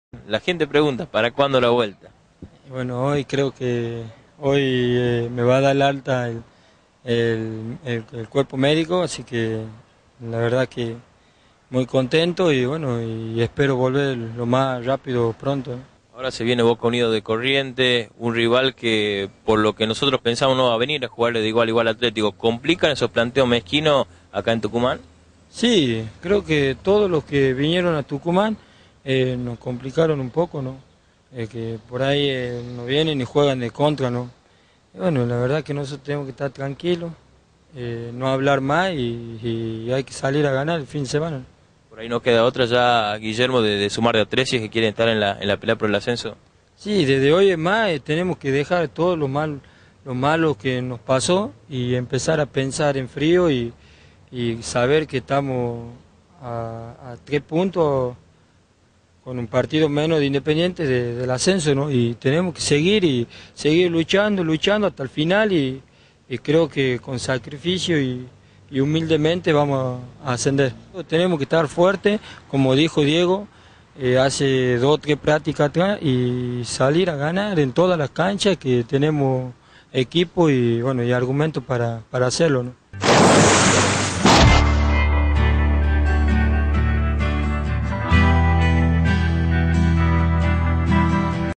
GUILLERMO ACOSTA
" Nosotros tenemos que estar tranquilos, no hablar màs y salir a ganar el fin de semana ", dice Guillermo Acosta. MIRA LA NOTA